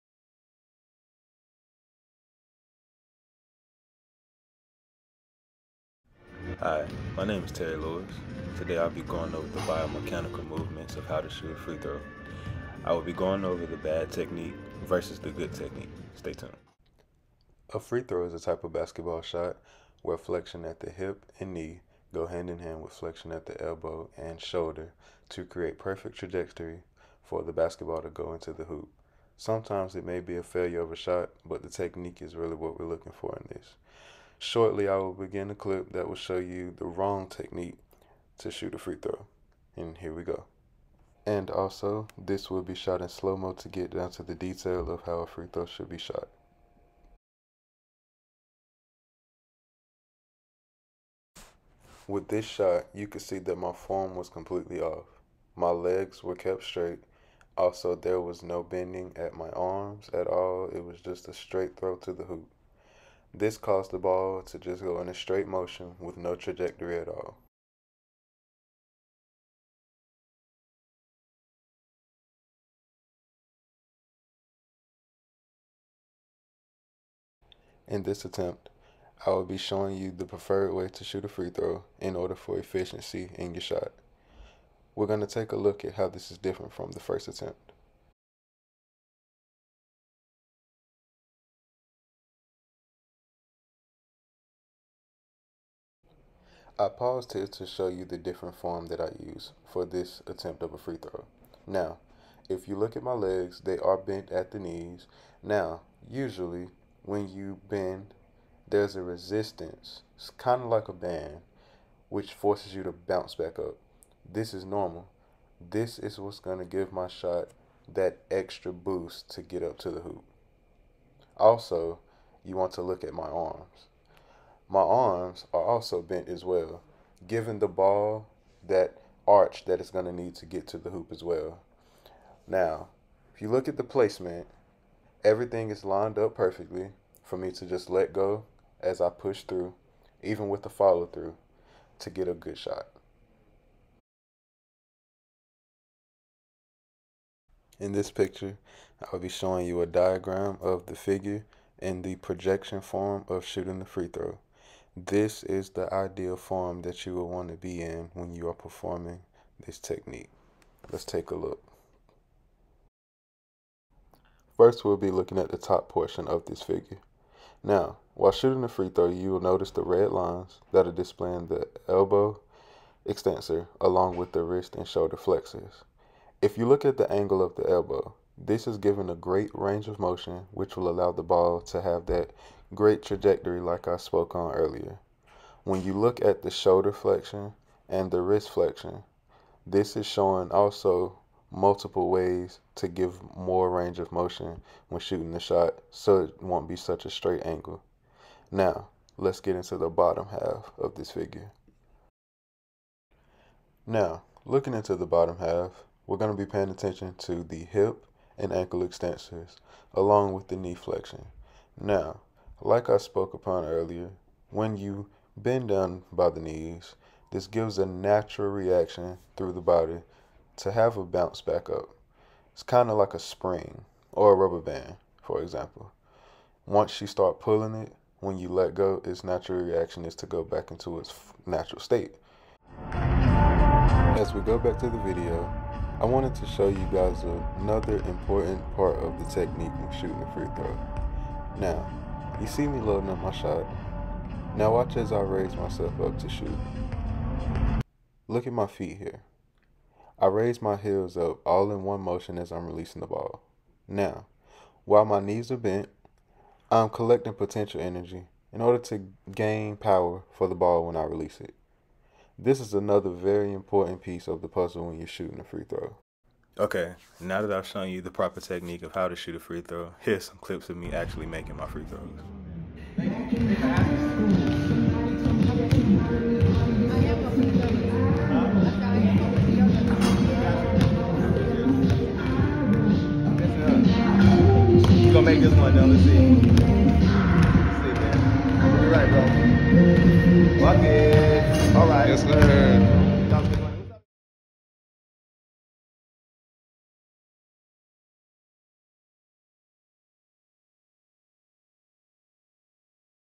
KIN 376 Applied Biomechanics
This video will be an explanation along with demonstrations of the proper technique of a free throw for efficiency.
Citation for Picture Used: Kim, Y.-K., Jung, J.-W., &amp; Park, C.-H. (2018). Joint kinematics and muscle activities to achieve ... Retrieved April 27, 2022,